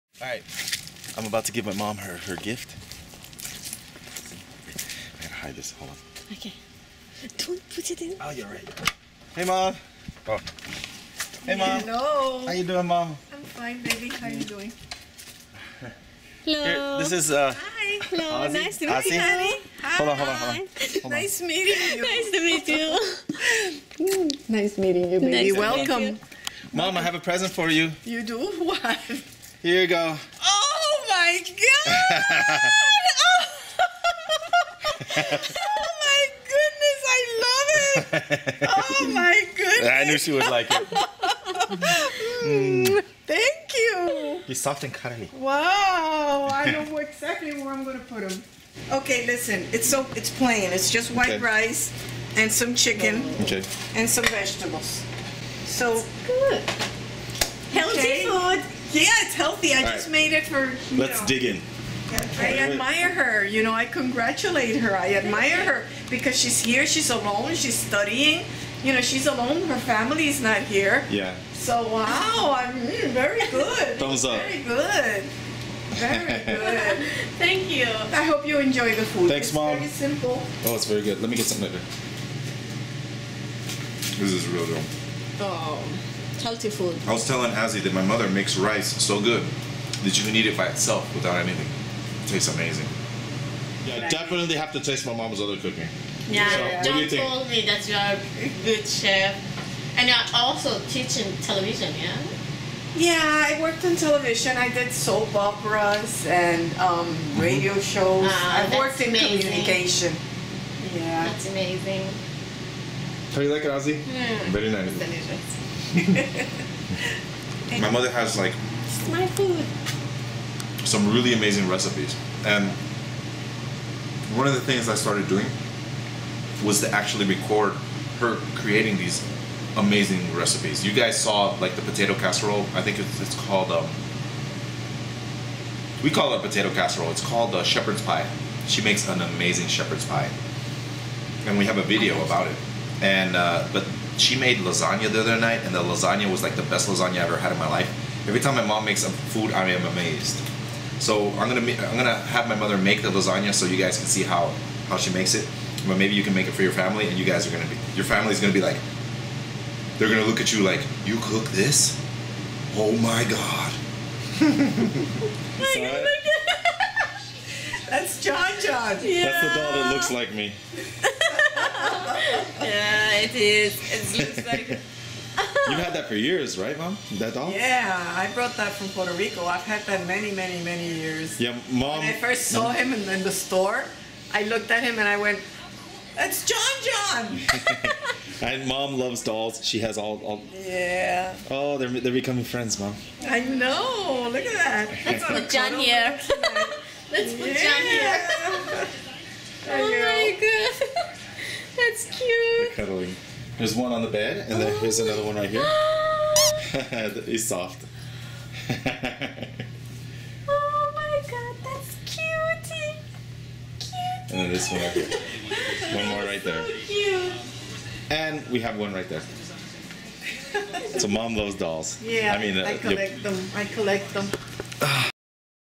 Azi tries my moms cooking
I start the day by giving my mom her gift i bought for her at them mall and then later on we eat dinner and Azi tries my moms cooking for the first time.